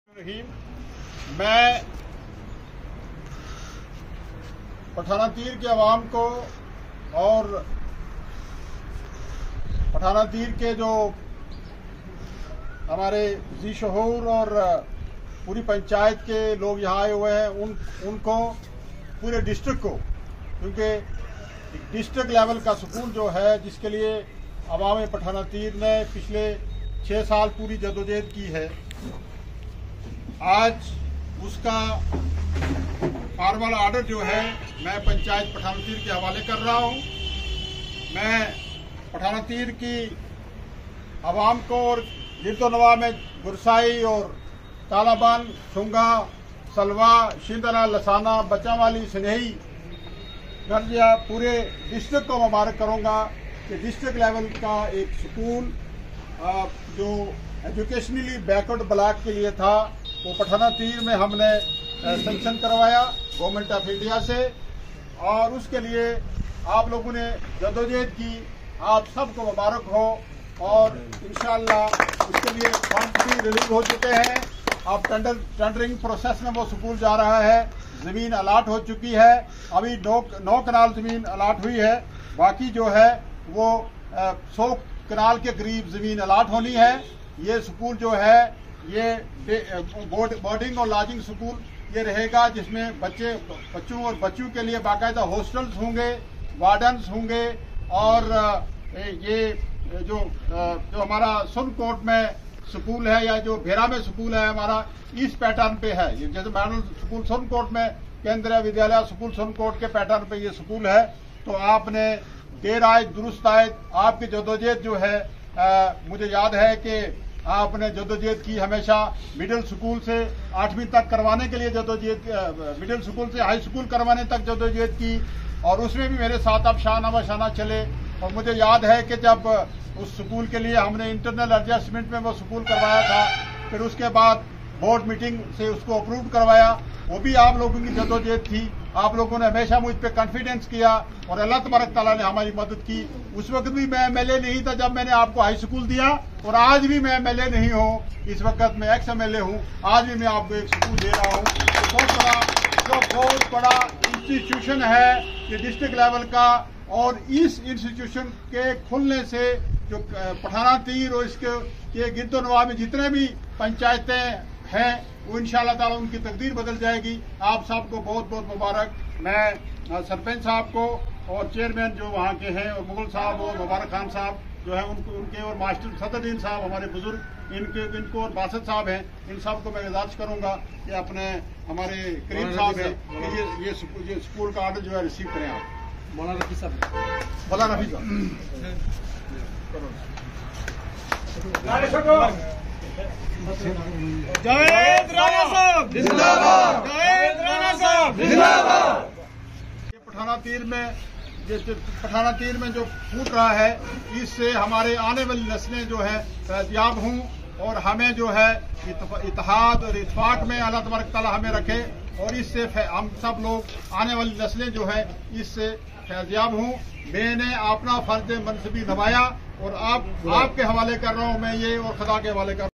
Javed A Rana former MLA mendhar handed over the land allotment order
Javed A Rana former MLA mendhar handed over the land allotment order for Model school patanatir for educationally   backward block sanction by Government of India to public of patanatir in presence of shaban Khan Sarpanch Patanatir today at Mendhar